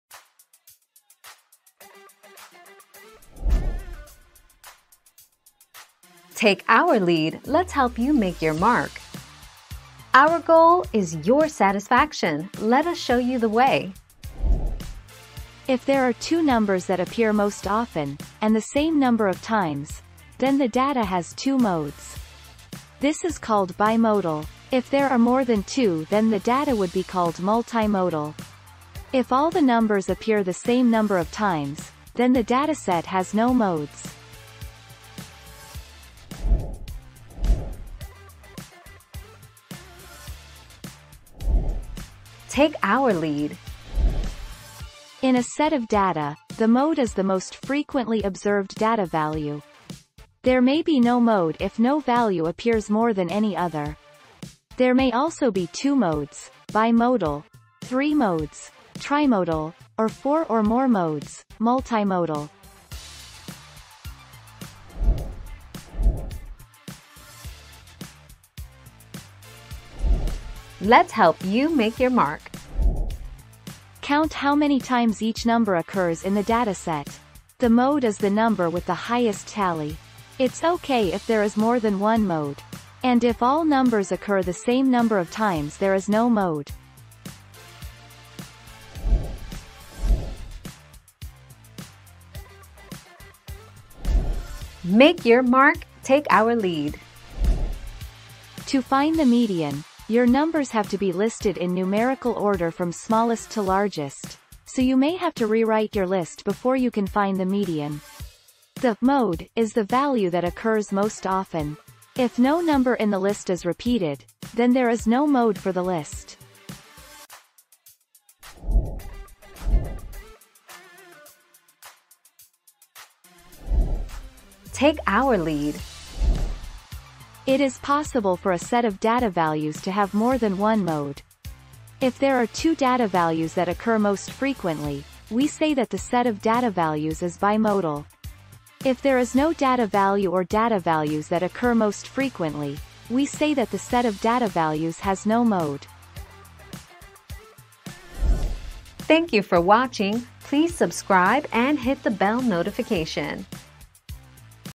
How do you find the mode if there are two?
00:00 - How do you find the mode if there are two?
00:38 - Can you have 3 modes?
01:06 - How do you find the mode on a calculator?
01:30 - How do you find the mode if no number is repeated?
02:00 - What if there is no mode?

Laura S. Harris (2021, April 6.) How do you find the mode if there are two?
    AskAbout.video/articles/How-do-you-find-the-mode-if-there-are-two-209810